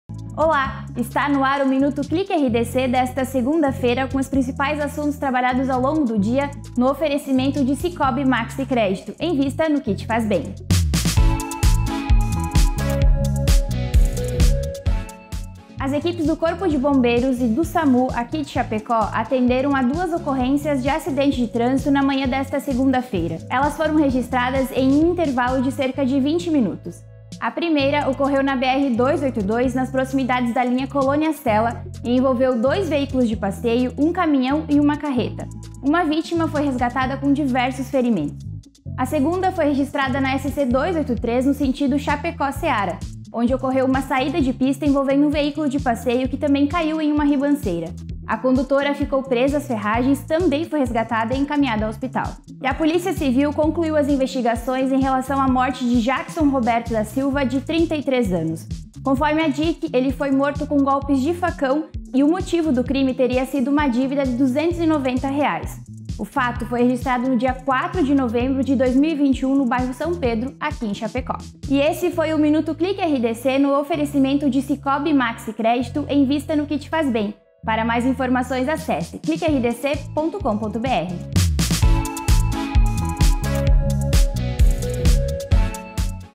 Minuto ClicRDC - 14/02/2022
00:00 - Bem-vindo ao Minuto ClicRDC
00:15 - Corpo de bombeiros atende a dois acidentes em 20 minutos
00:48 - Concluída investigação da morte de Jackson Roberto da Silva
01:11 - Acesse clicrdc.com.br

Patrocínio: Sicoob MaxiCrédito, invista no que te faz bem!